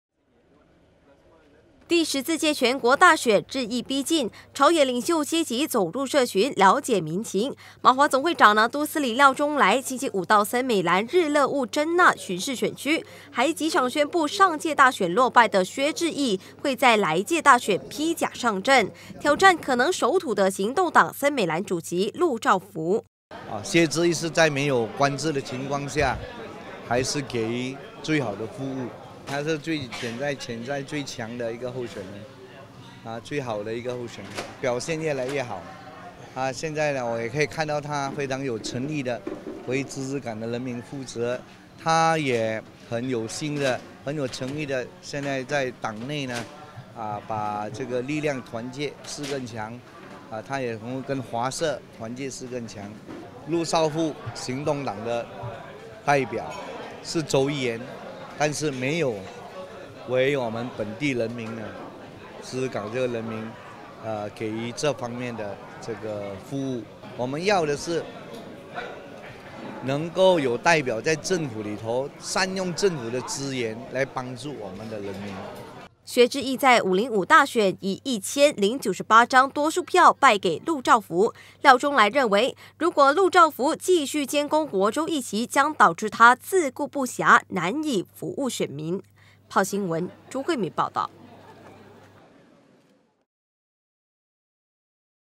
16/12: 倒数全国大选 廖中莱再宣布上阵人选
第14届全国大选脚步逼近，马华总会长廖中莱陆续宣布上阵人选。